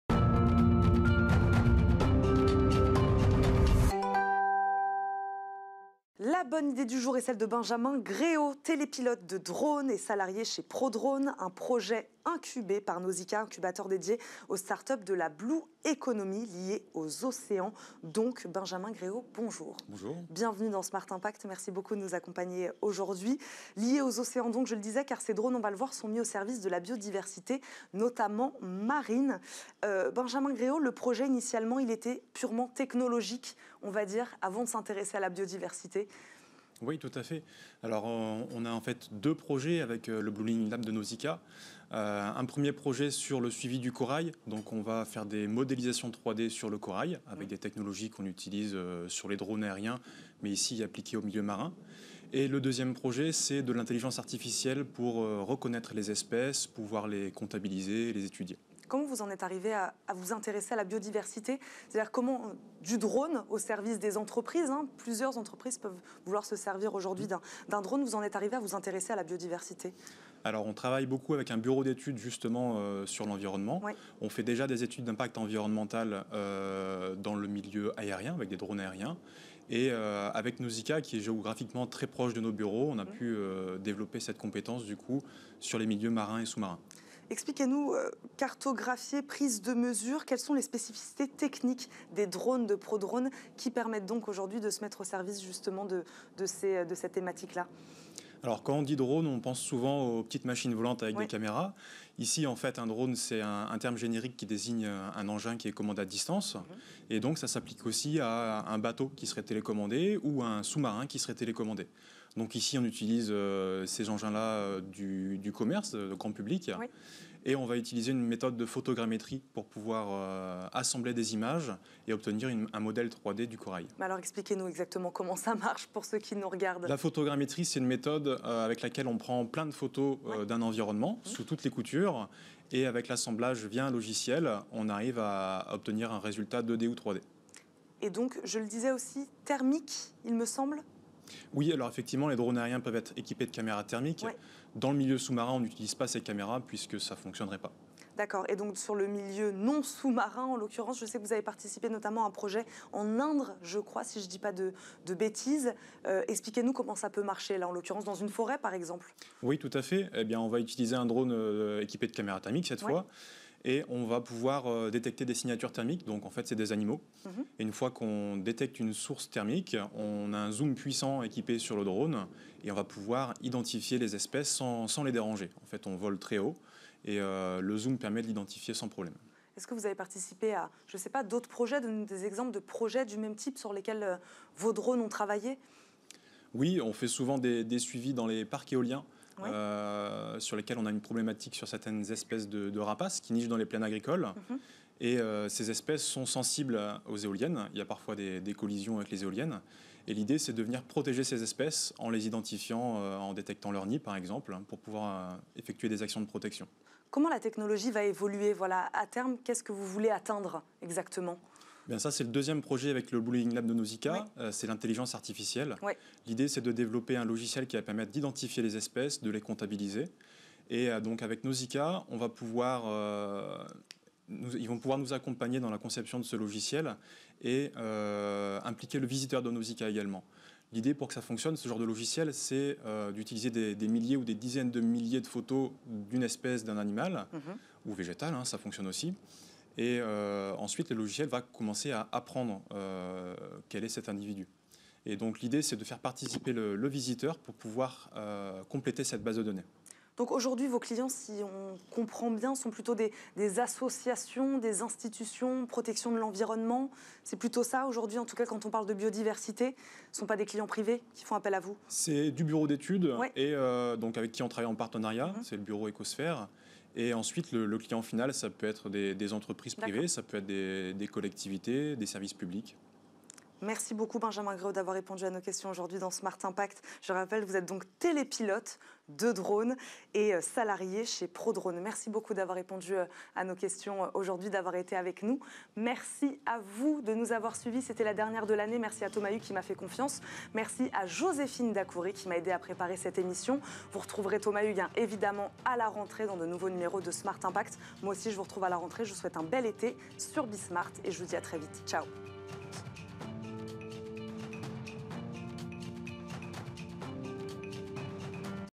SMART IMPACT - Smart Ideas du vendredi 22 juillet 2022
Vendredi 22 juillet 2022, SMART IMPACT reçoit Benjamin Gréau (Télépilote de drones, Prodrones)